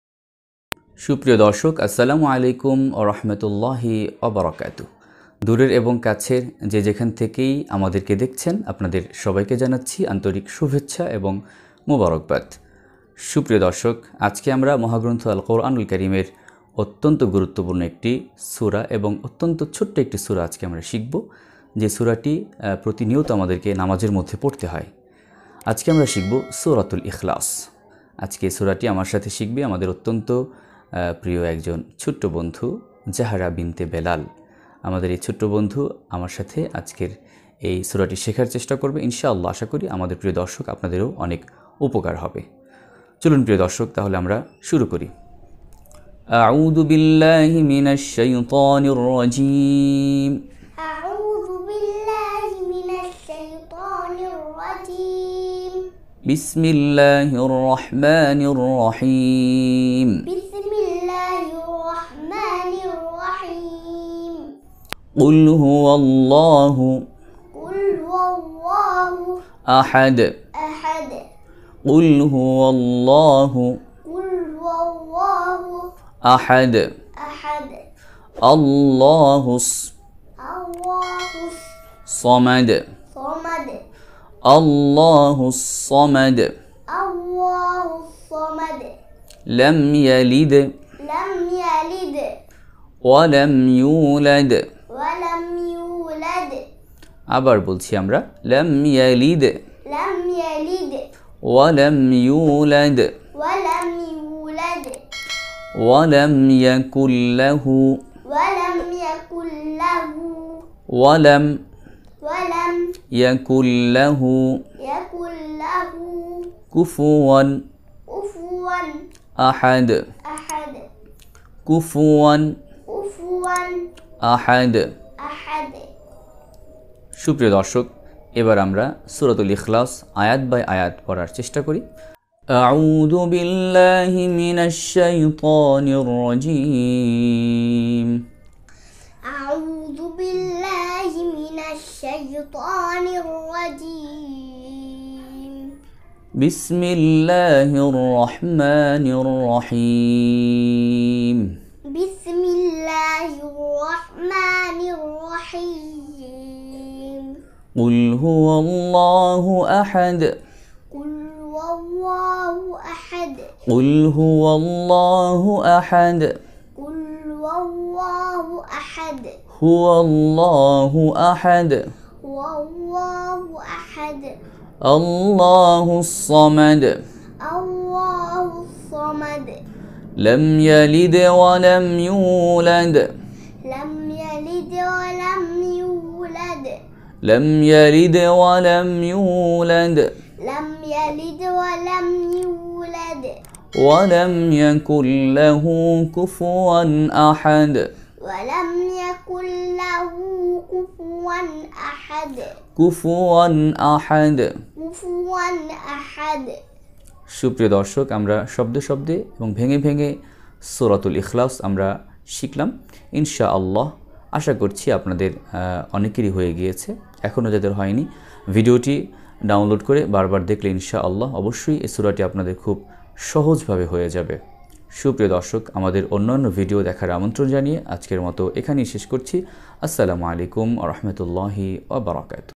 এতো ছোটো মেয়েকে হুজুর কি শিখাচ্ছেন? surah ikhlas bangla | quran shikkha bangla uccharon
সূরা ইখলাস বাংলা উচ্চারণ সহ খুব সহজে শিখুন।
আলোচকঃ
মাওলানা নাঈম বিন আমিন ও জাহরা বিনতে বেলাল।

আপনার সন্তানকে কোরআন শিক্ষা দিতে চাইলে যোগাযোগ করুনঃ